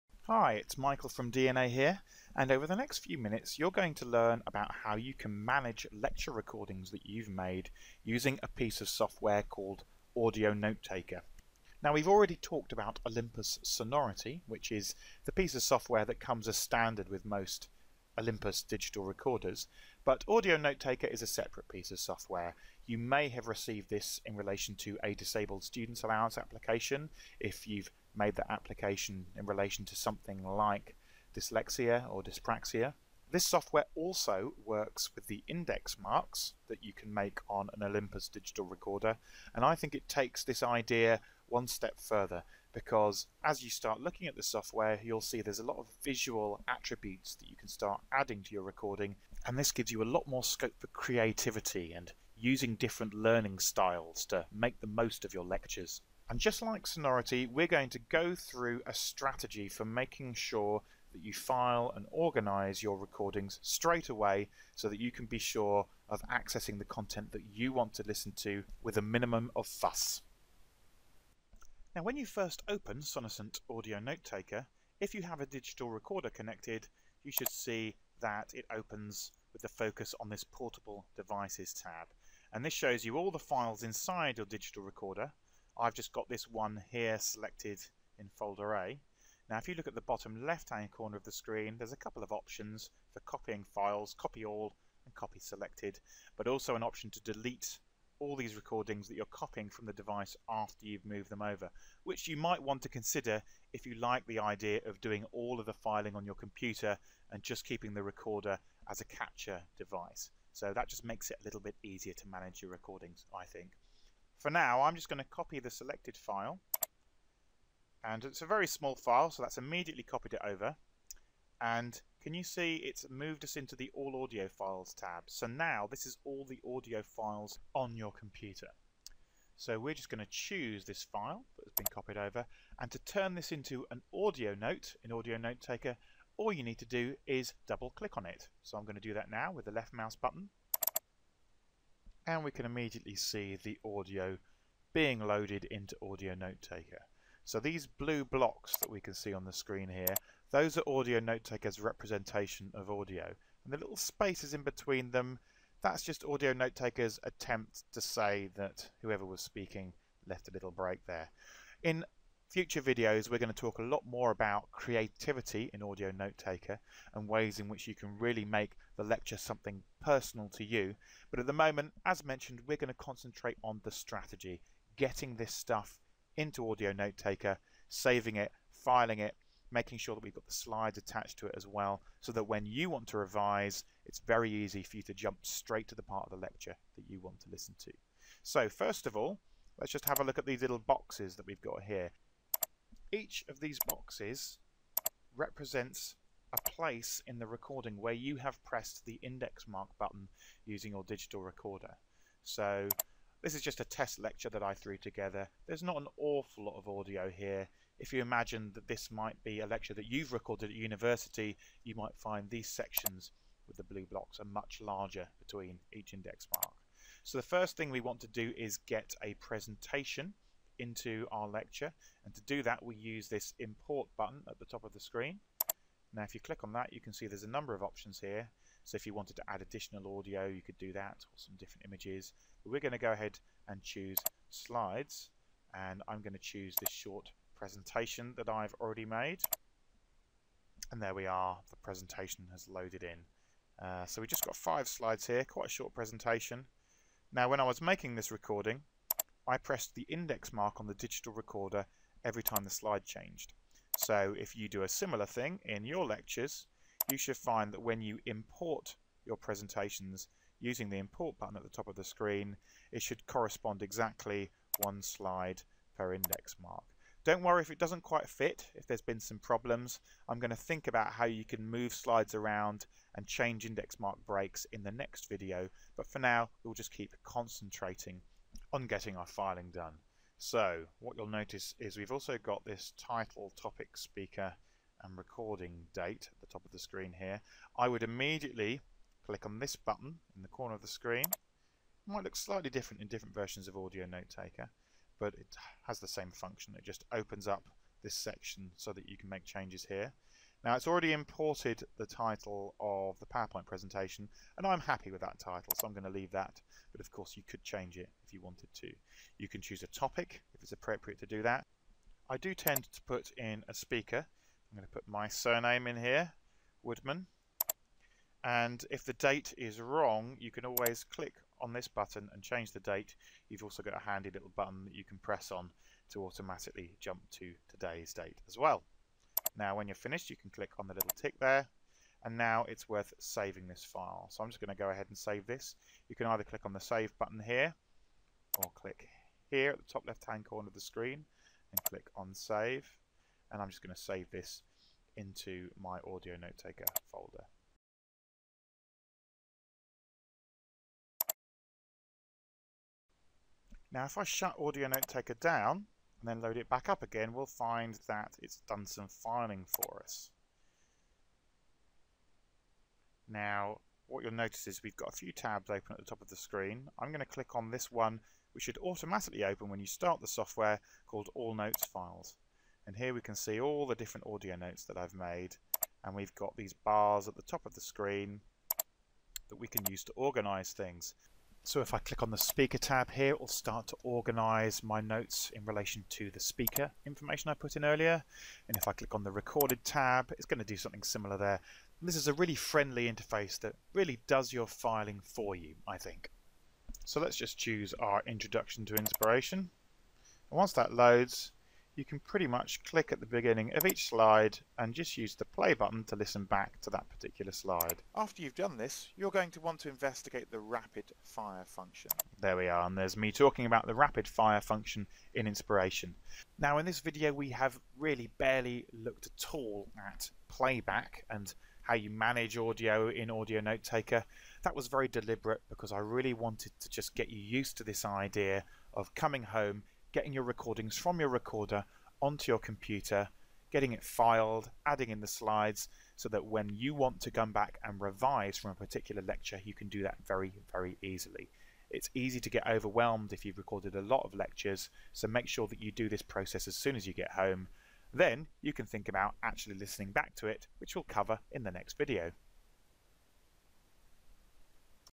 5: Managing your recordings with Sonocent Audionotetaker.
Copying audio files from a recording device, and archiving these in Sonocent's Audionotetaker software.